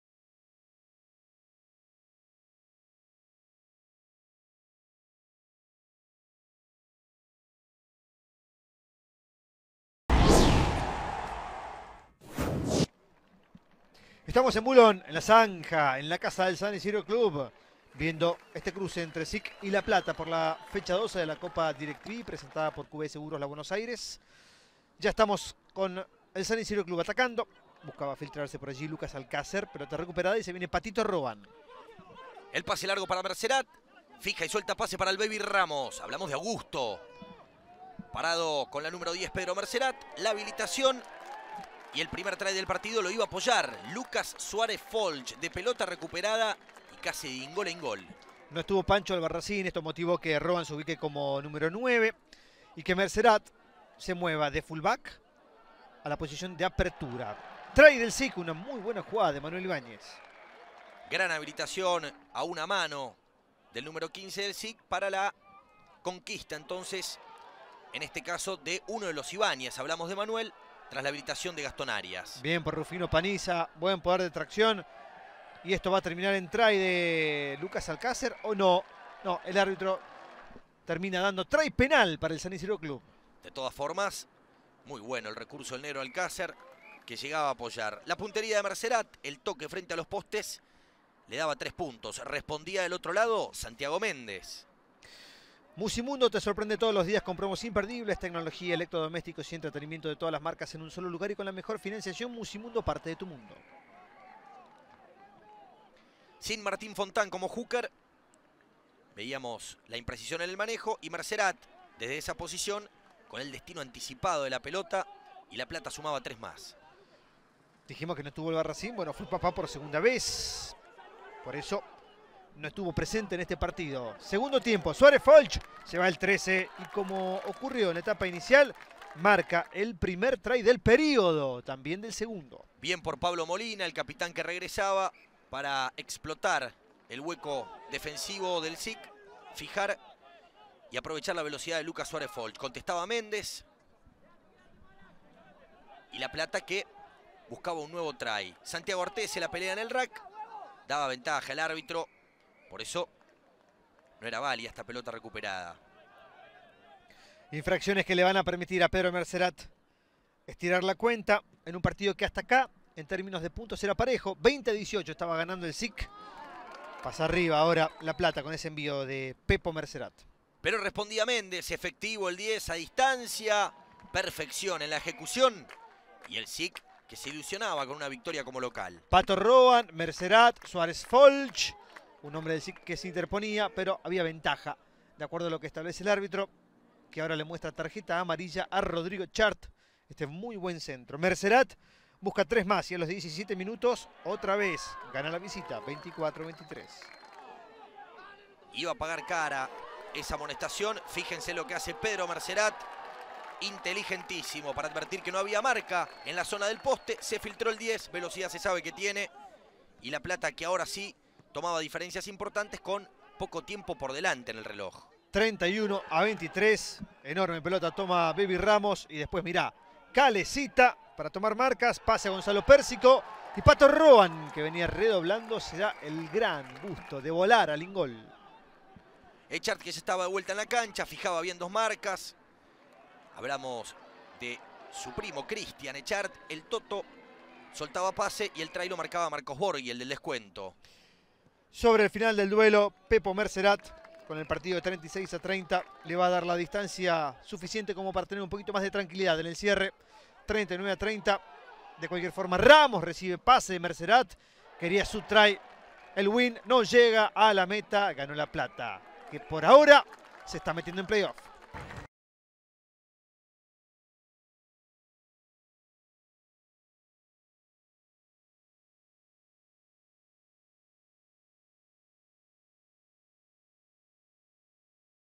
SIC vs La Plata - Top 14 - Resumen ESPN
Publicamos el resumen de ESPN del partido con La Plata, por la décimo segunda fecha del top 14.